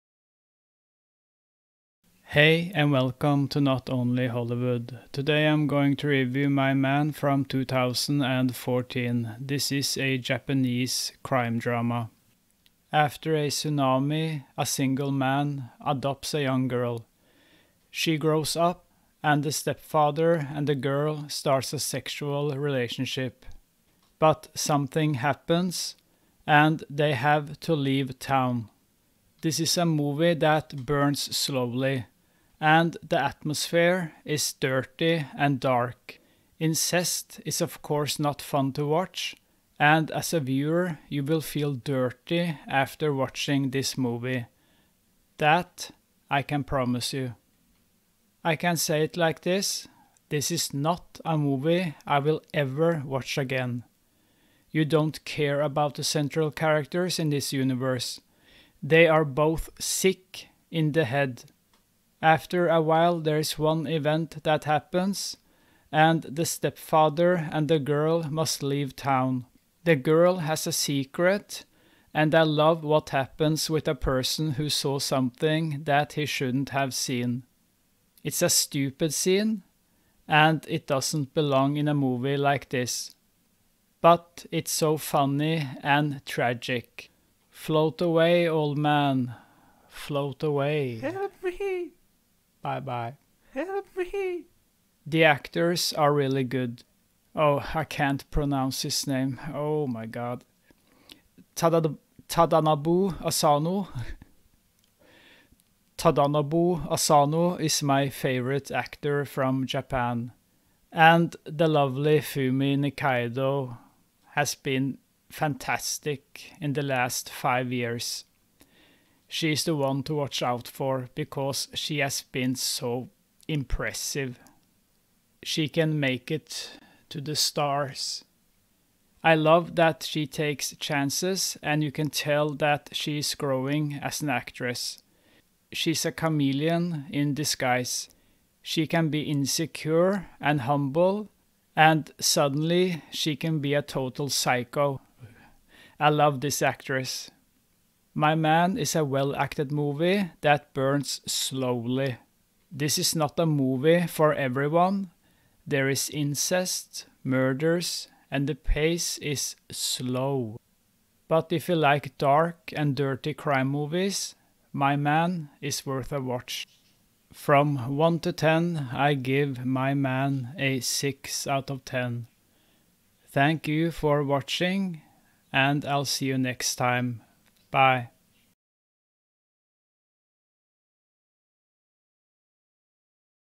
My Man (2014) | Movie Review | Japan | Forbidden love between two of Japans best actors
Movie review of My Man (2014). This is a Japanese crime-drama about a stepfather and his young daughter who loves each other too much, and they do a forbidden thing.

This is a dark movie about love and crime. It has two of the best actors in Japan, Fumi Nikaido and Tadanobu Asano. So there's nothing wrong with the acting!